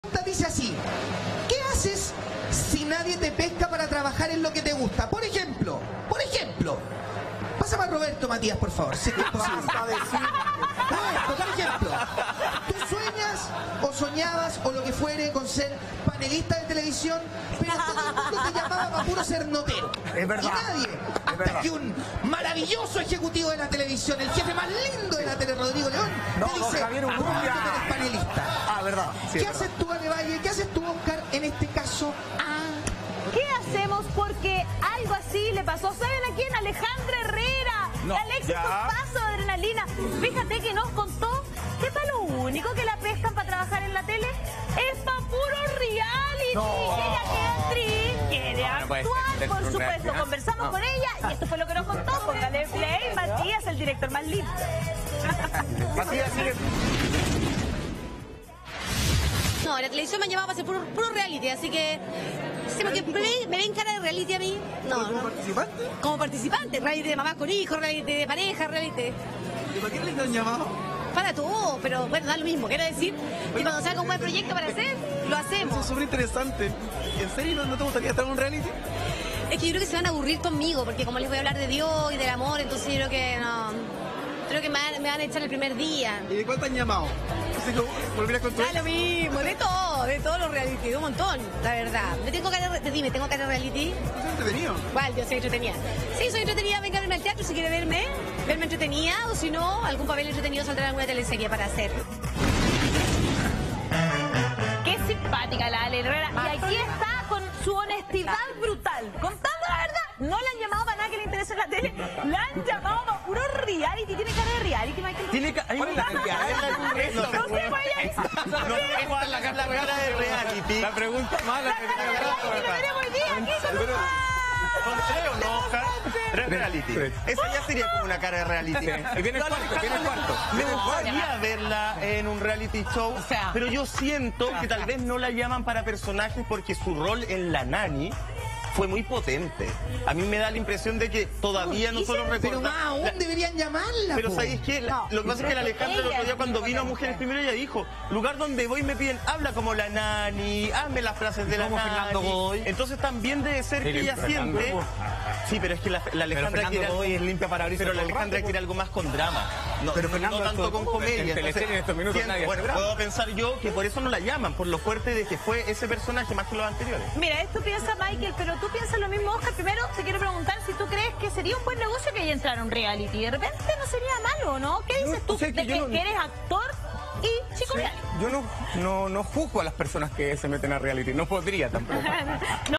Las declaraciones de Alejandra Herrera sobre ofertas laborales que la han hecho
La actriz confesó que ha recibido muchas propuestas de trabajo, pero no en lo que ella quiere desarrollarse.